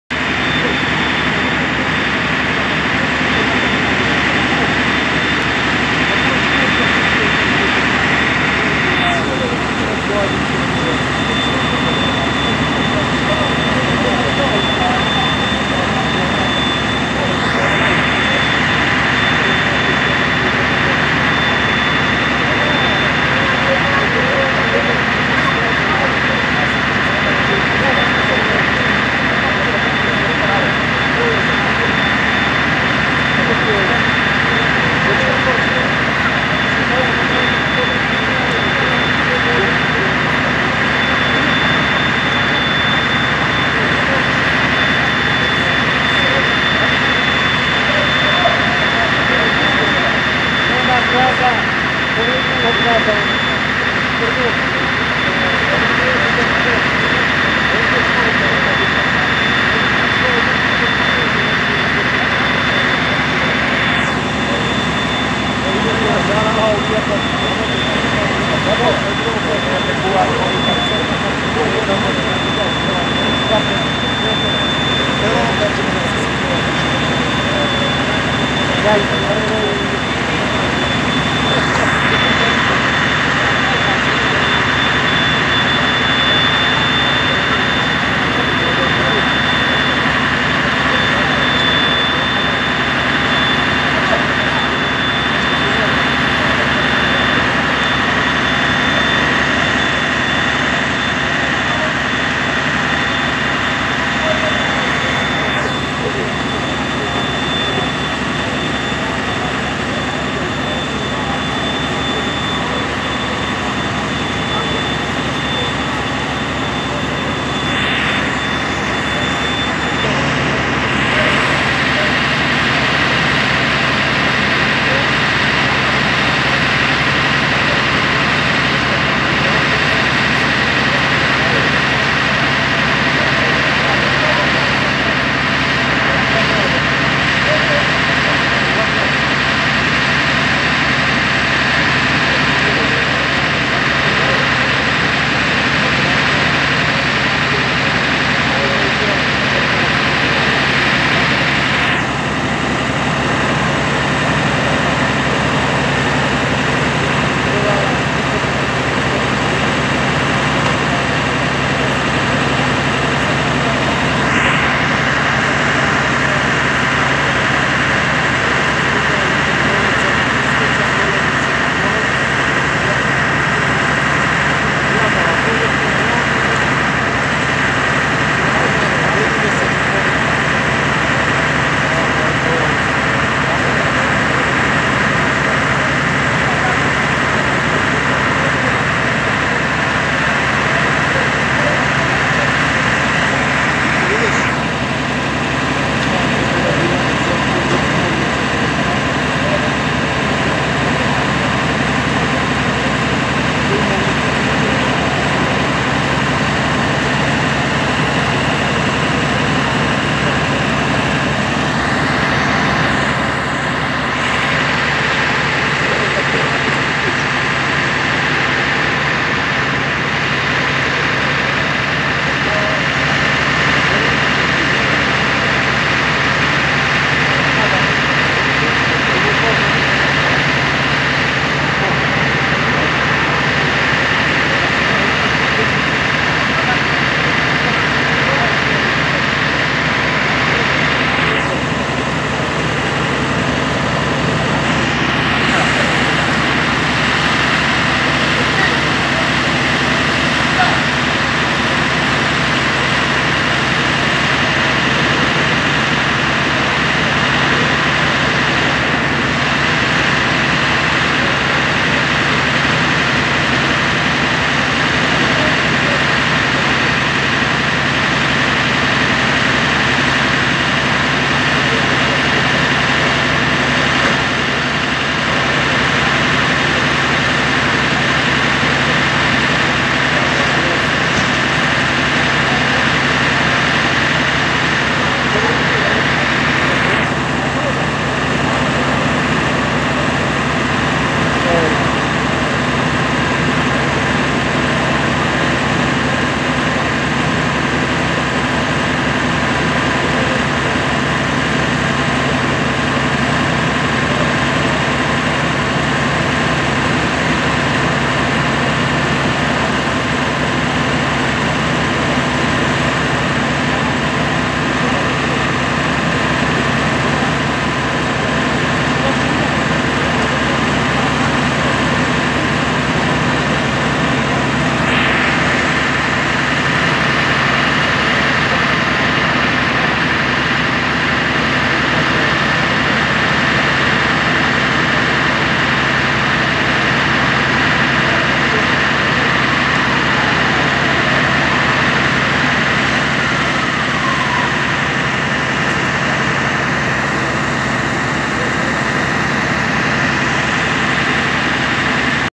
Pozar vo Ministerstvo za pravda vo Skopje Makedonija
Пожарот, во кој се зафатени канцеларии во Министерството, е пријавен во 16 часот и 45 минути.

Во гаснење на пожарнот интервенираат 10 возила со 25 пожарникари.